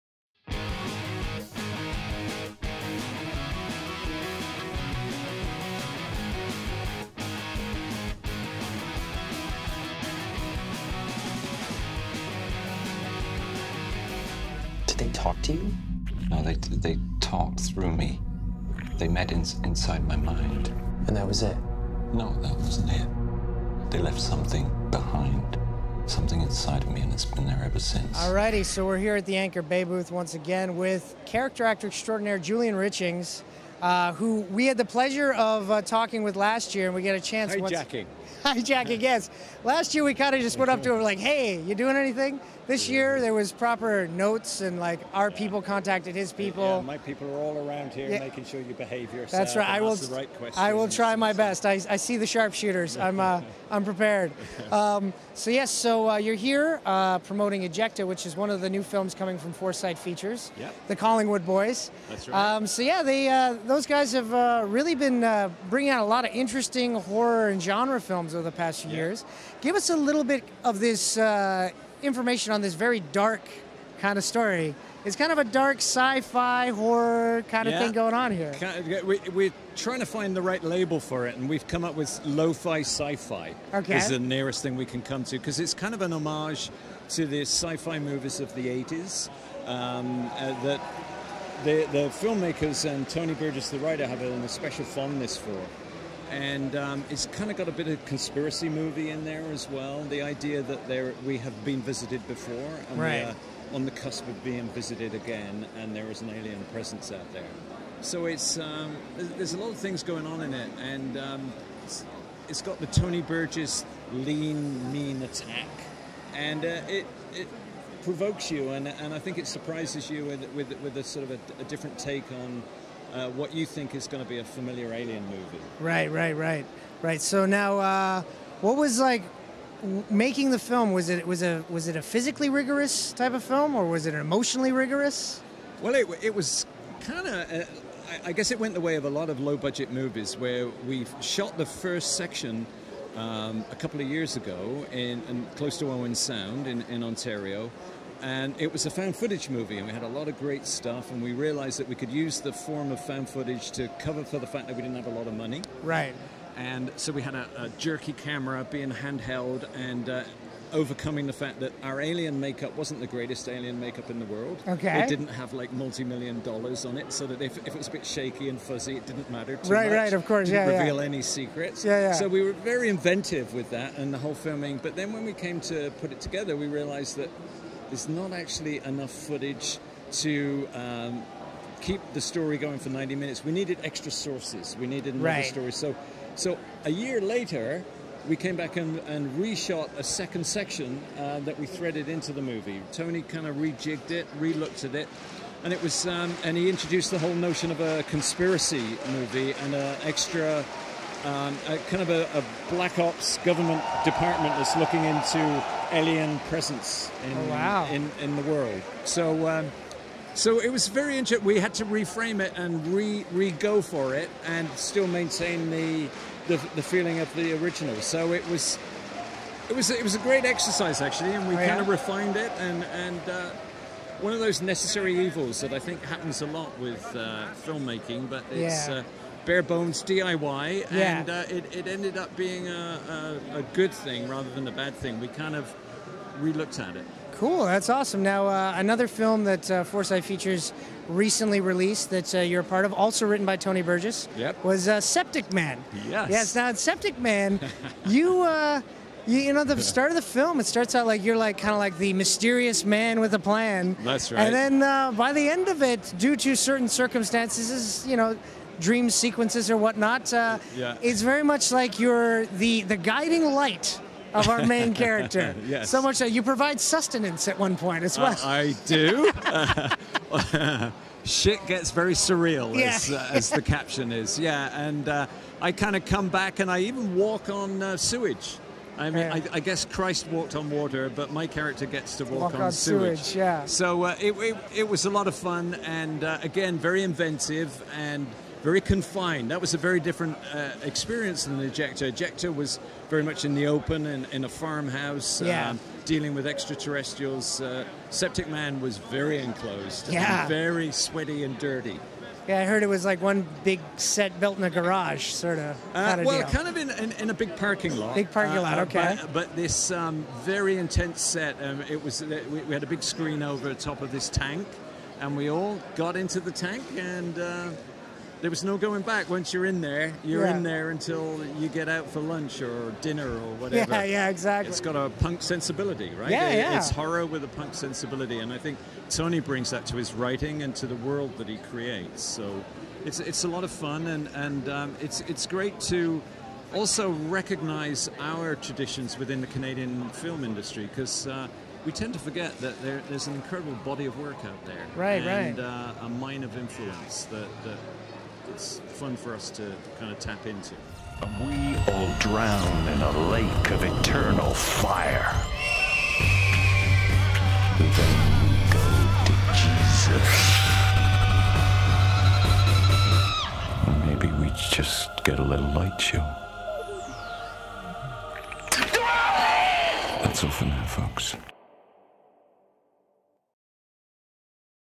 Geek Hard Presents: One on One with Julian Richings
This past summer at Fan Expo Canada, for the second year in row, Andrew got a chance to chat with actor Julian Richings at the Anchor Bay Booth. But unlike last year, this interview was planned in advance. Watch as Julian talks about low budget horror and his work with the cats from Foresight Features on the films Ejecta and Septic Man.